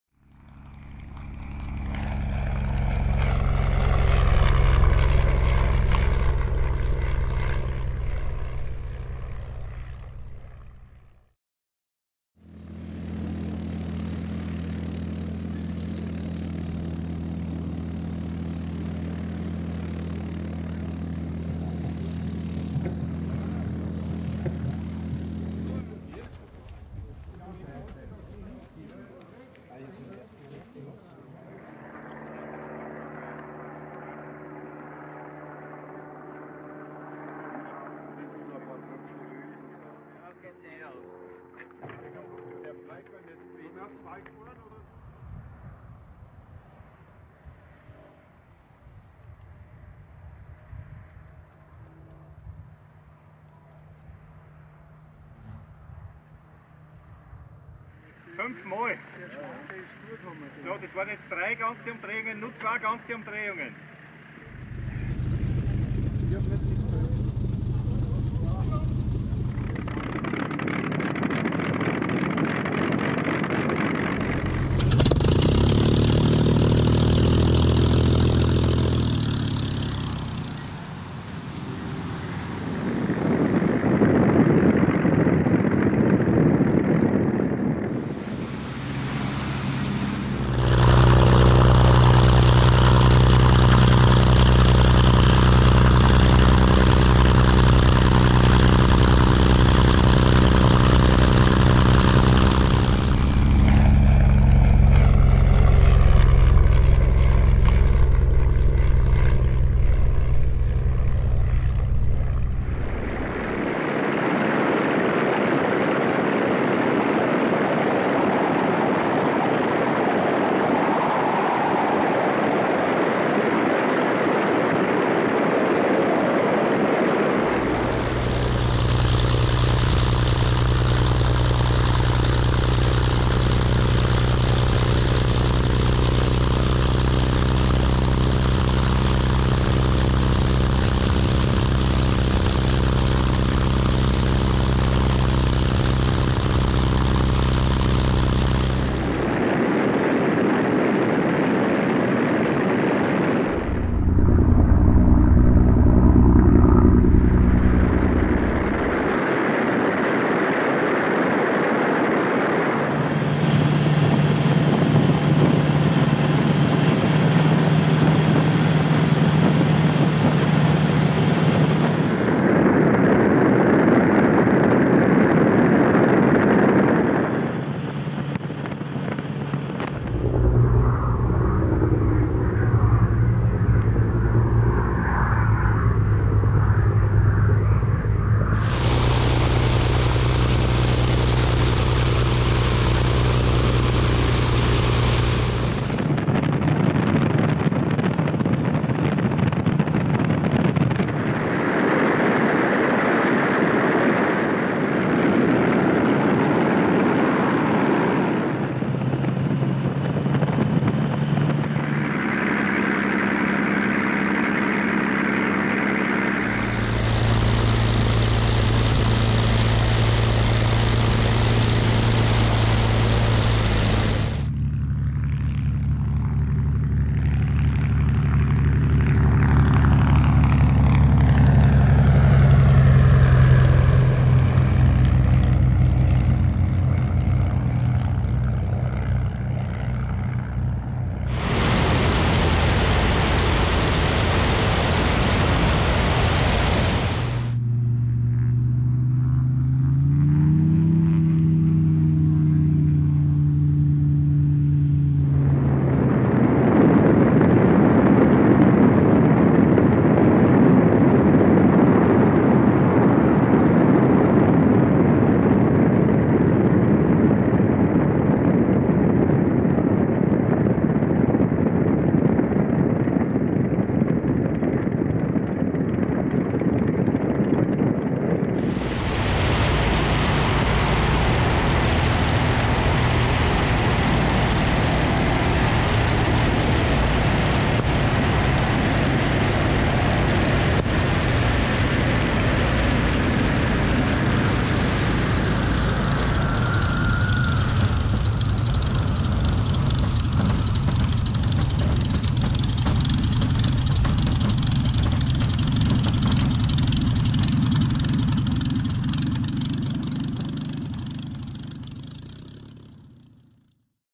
Albatros DIII & Hansabrandenburg CI
Die Beiden einzigen flugfähigen Flugzeuge der K&K Luftfahrtruppen gemeinsam in der Luft!
Ein Jahrhundertereignis!!